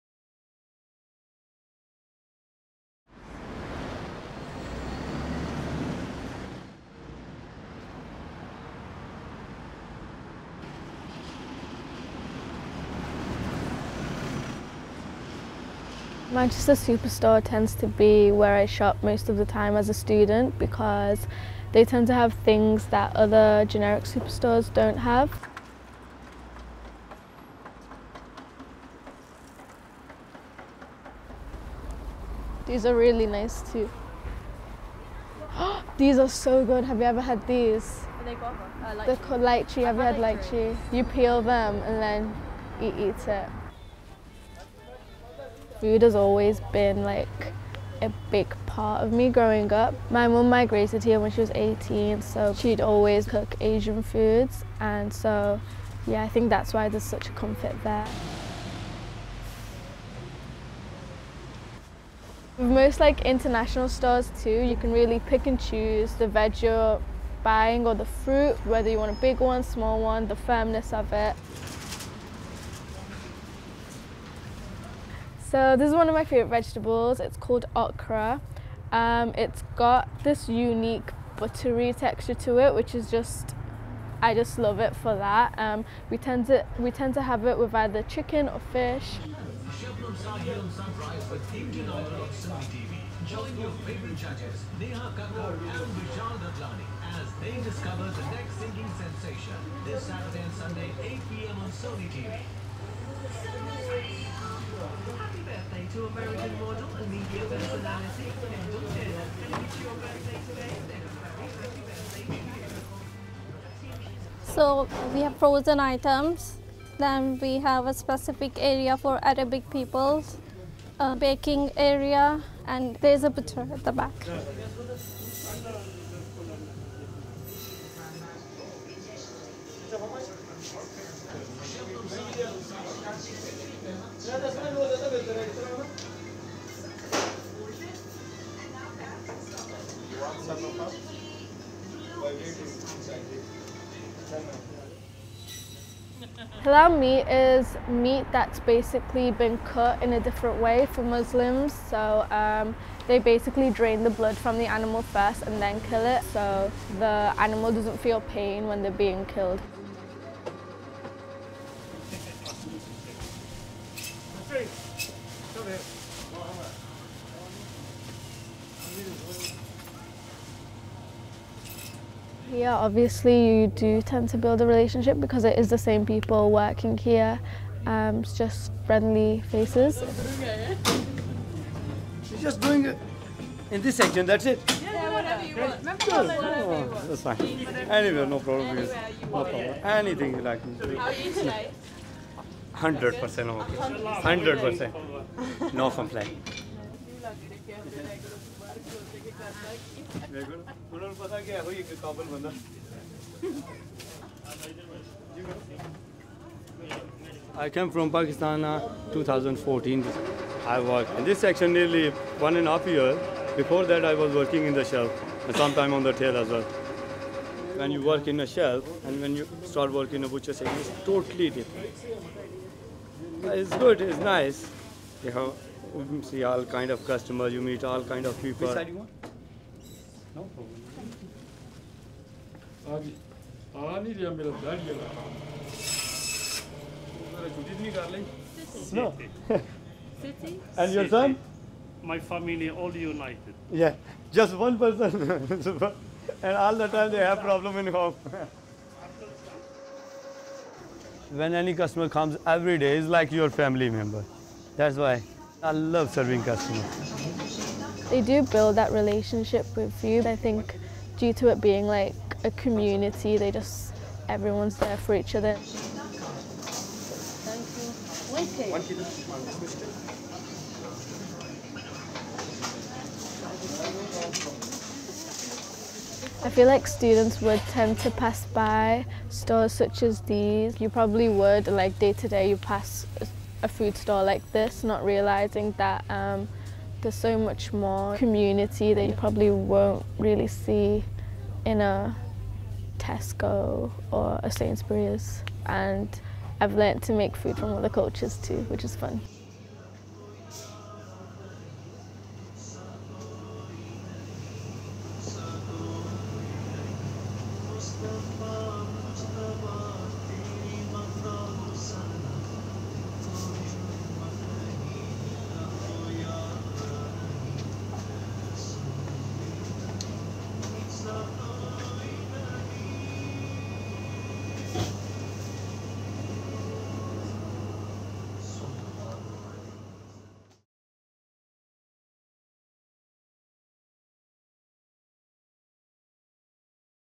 Next Door's Superstore (2022) | Short Documentary
A film by George Higgins, Uma Carey-Morgan, Harrison Gill, and Molly Zazzera, for the Documentary course at Manchester University.